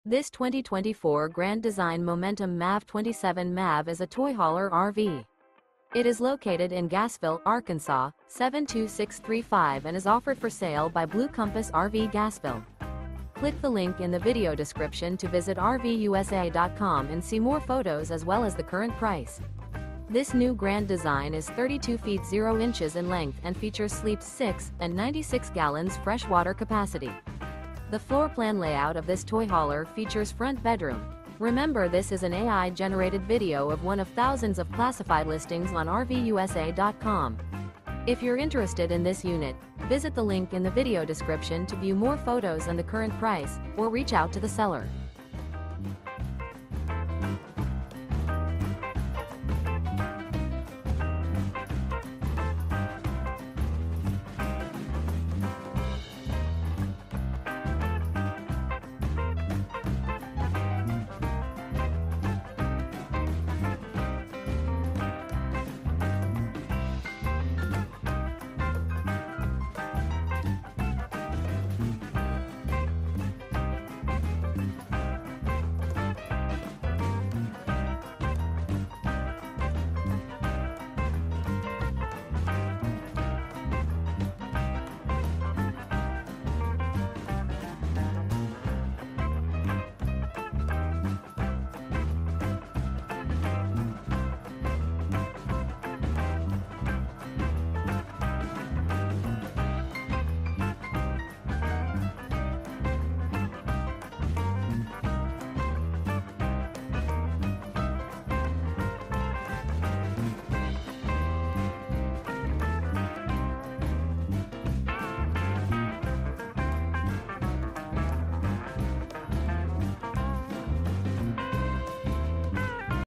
Remarkable 2024 Grand Design Momentum Toy Hauler RV For Sale in Gassville, AR | RVUSA.com
This 2024 Grand Design Momentum MAV 27MAV is a Toy Hauler RV. It is located in Gassville, AR 72635 and is offered for sale by Blue Compass RV Gassville. This New Grand Design Is 32' 0" in length and features sleeps 6, and 96 gal fresh water capacity. The floorplan layout of this Toy Hauler features Front Bedroom.